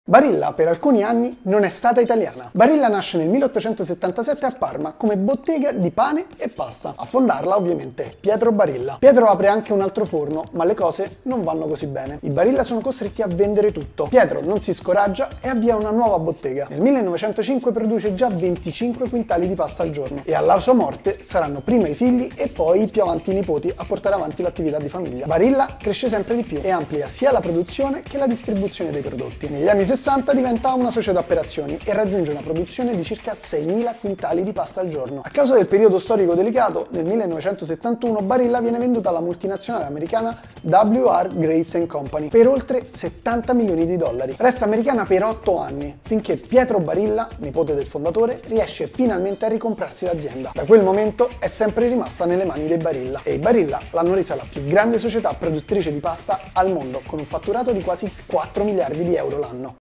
Barilla per alcuni anni è stata americana
Gli italiani consumano 23 kg di pasta pro capite all’anno.

Lo rivela una ricerca di Unione italiana food. Dopo di noi ci sono Tunisia con 17 kg, Venezuela con 15 kg e Grecia con 12,2 kg pro capite.

Quante volta alla settimana mangi pasta?

Testi: Martina Ferraro
Montaggio: Maicol Morbidelli
Coordinamento: Edoardo Scirè